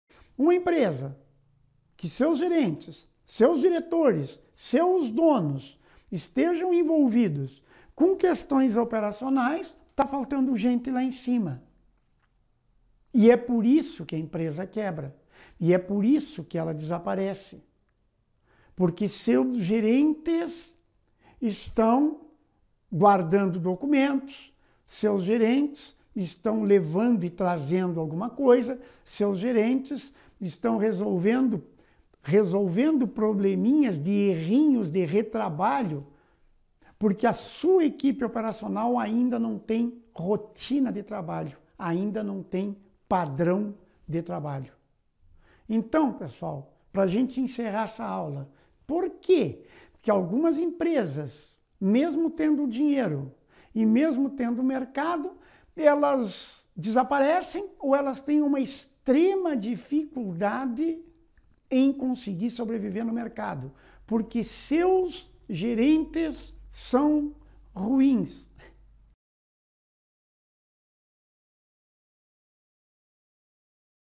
VOCÊ É UM GERENTE DE VERDADE?
Na aula desta semana do Curso Virtual Gratuito de Liderança e Gestão o professor Vinicius Paiva da Silva fala por que algumas empresas não dão certo, mesmo tendo dinheiro e mercado.